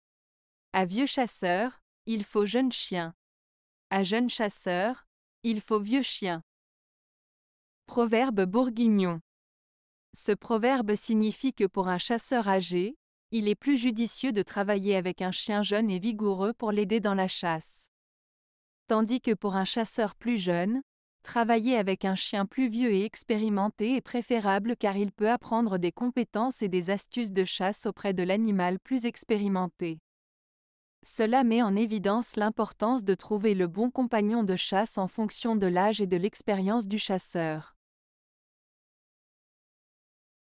À vieux chasseur, il faut jeune chien.À jeune chasseur, il faut vieux chien. - Proverbe Bourguignon
Ce proverbe signifie que pour un chasseur âgé, il est plus judicieux de travailler avec un chien jeune et vigoureux pour l'aider dans la chasse. Tandis que pour un chasseur plus jeune, travailler avec un chien plus vieux et expérimenté est préférable car il peut apprendre des compétences et des astuces de chasse auprès de l'animal plus expérimenté. Cela met en évidence l'importance de trouver le bon compagnon de chasse en fonction de l'âge et de l'expérience du chasseur.